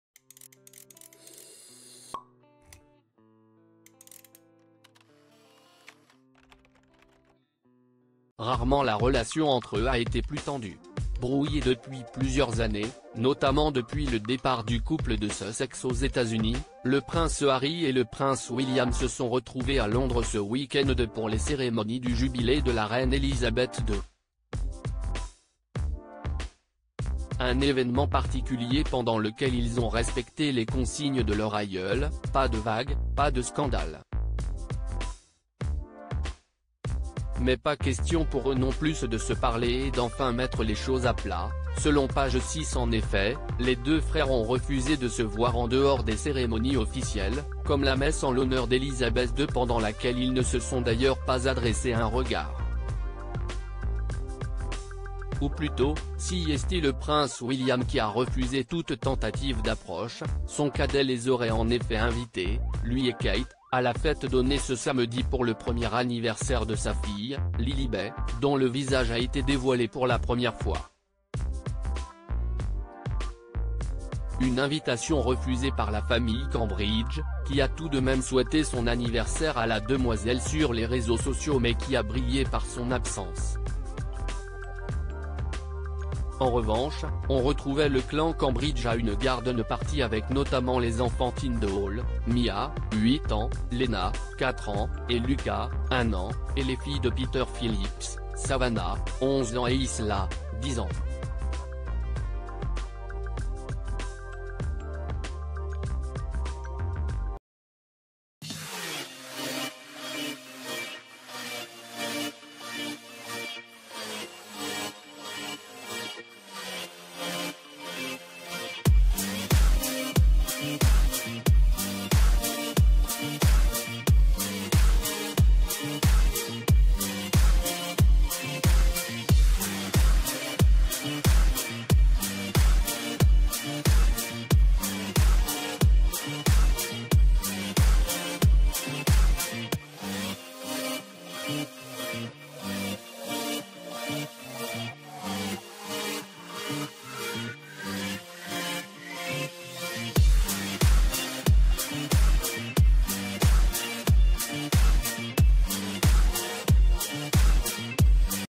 Kate Middleton et le prince William : Cette main tendue du prince Harry et de Meghan Markle qu'ils o
Kate Middleton et le prince William : Cette main tendue du prince Harry et de Meghan Markle qu'ils ont refusée...